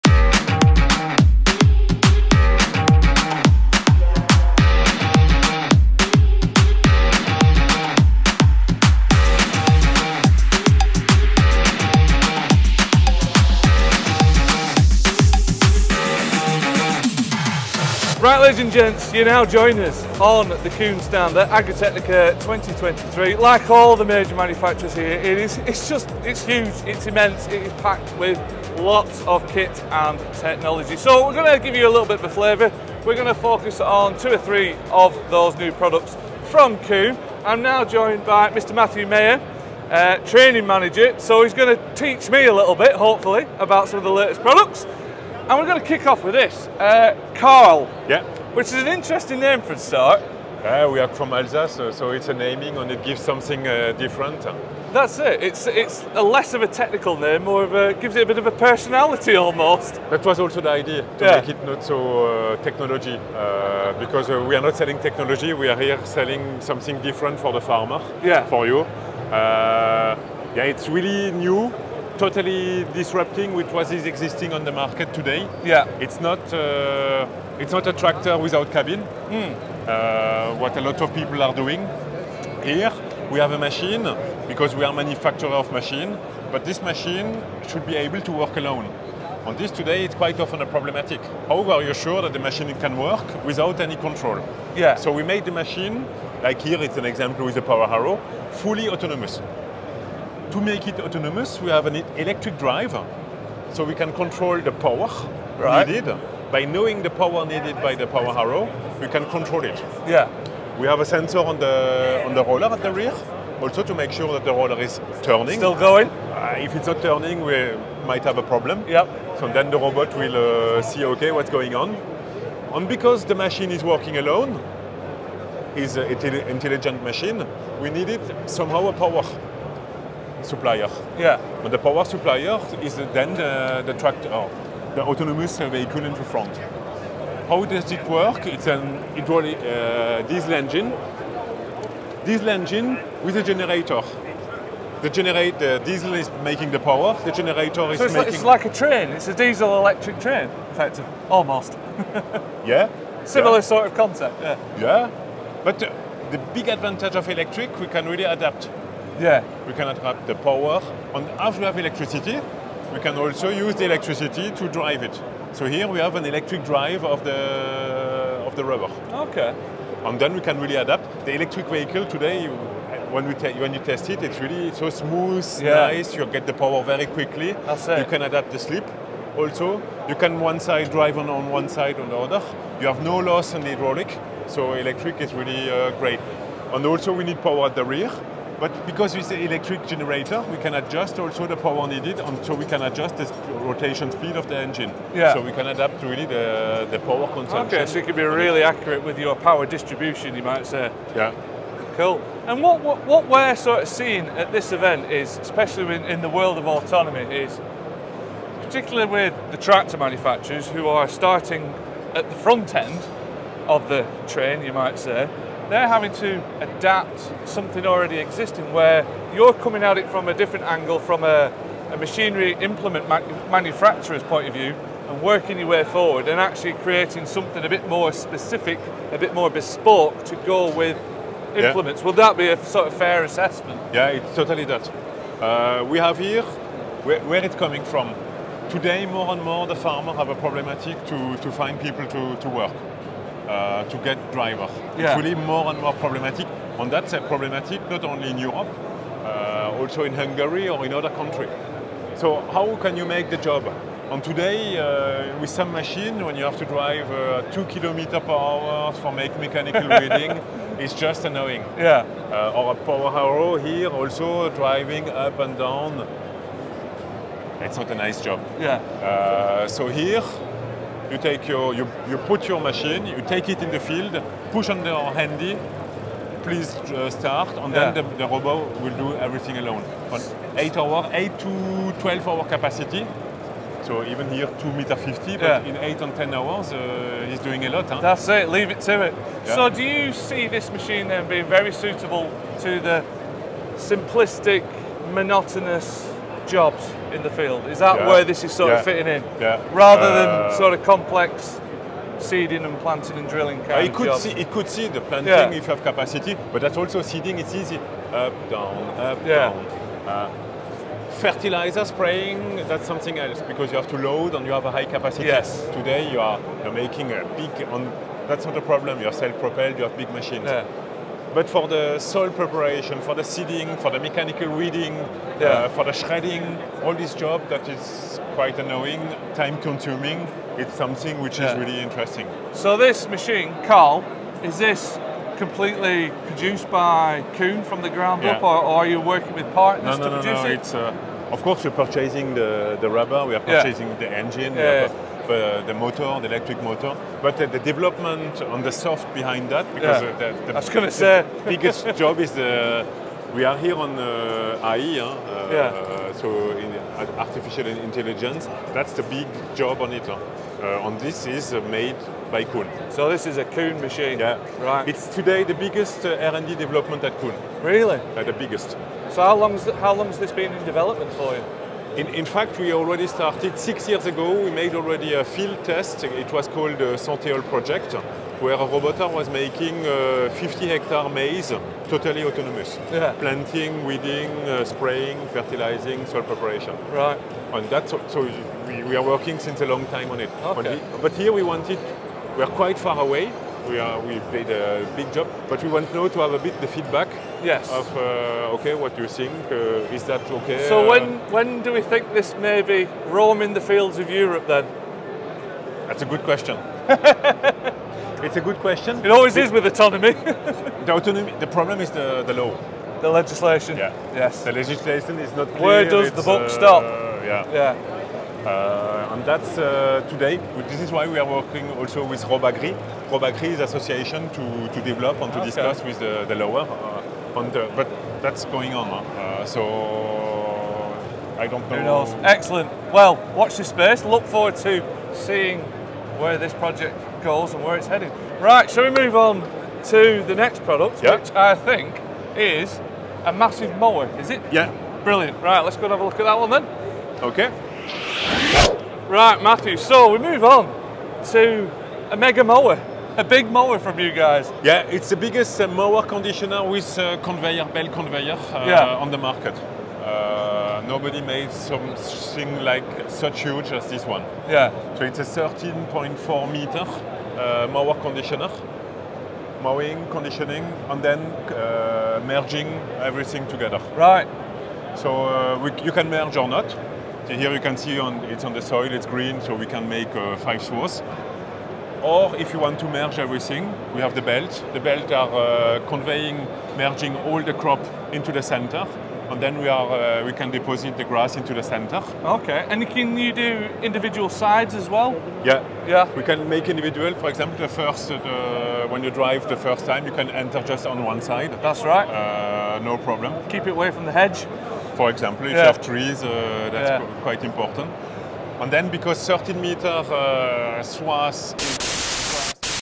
AGRITECHNICA 2023: Kuhn Autonomy, Massive Mowers and Clever Balers
There was no shortage of innovations on the Kuhn stand, as we checked out an autonomous vehicle, a massive mower and a clever combi-baler.
For all of our Agritechnica coverage, head on over to LandPowerTV.com